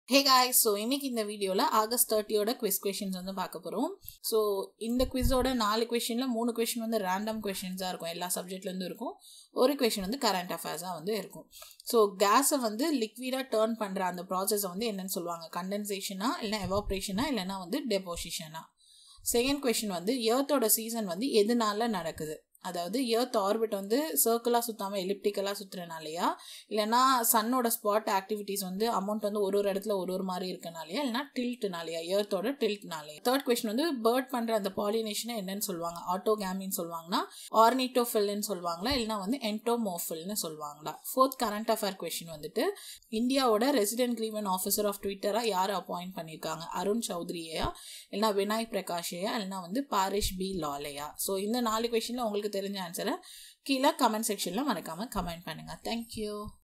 TNPSC/TET/TRB | Daily Quiz - August 30 (Comment ur answers) | Super Smart | Test Series
This short video is the quiz, in which 4 questions are given with options. Comment ur answers